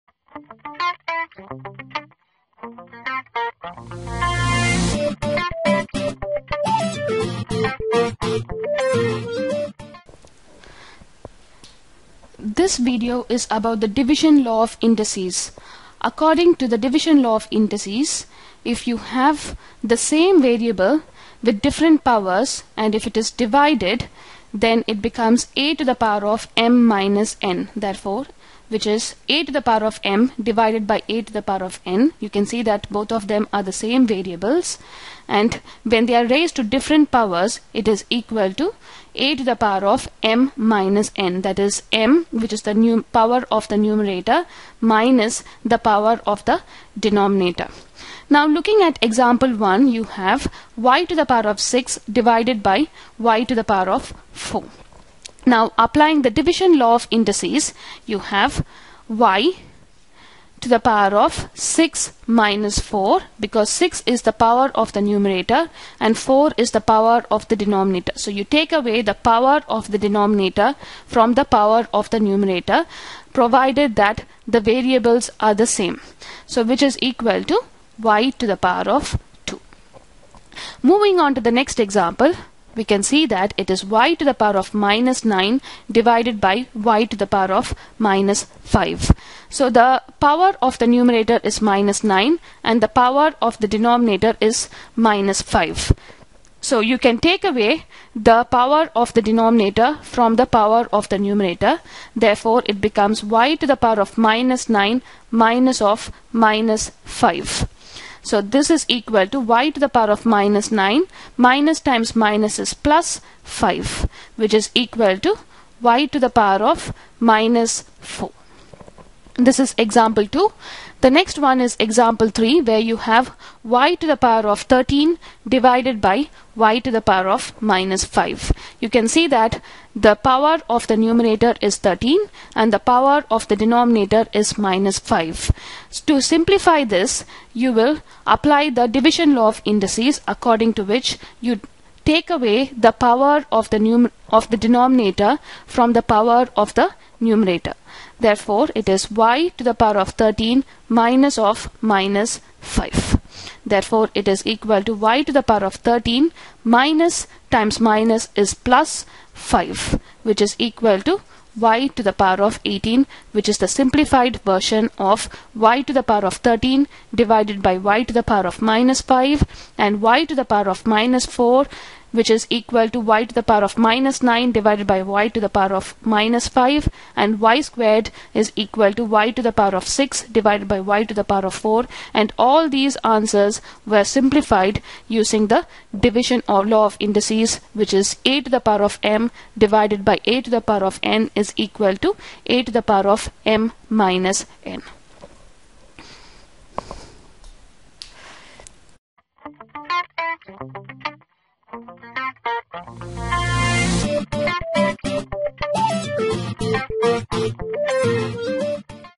Law of indices part 1
offers FREE Maths webinars. The video shows how to use law of indices to simplify algebraic expressions.